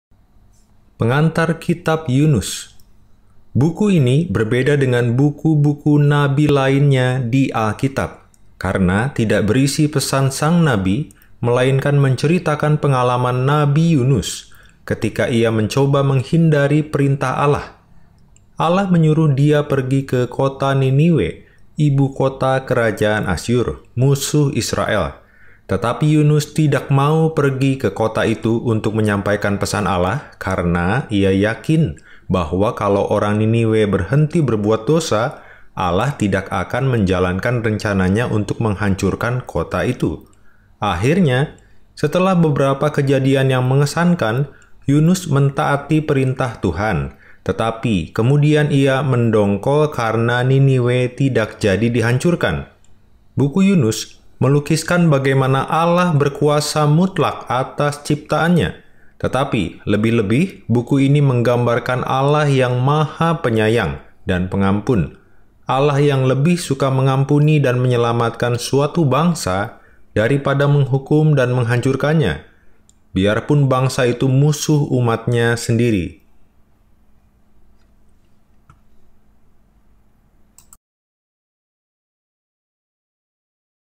PL-32 Pengantar Kitab Yunus - Seri Pengantar Alkitab
Yunus adalah seorang nabi Ibrani, putra Amittai, yang dikirim oleh Tuhan untuk menubuatkan kehancuran Niniwe, tetapi mencoba untuk melarikan diri dari misi ilahi ini.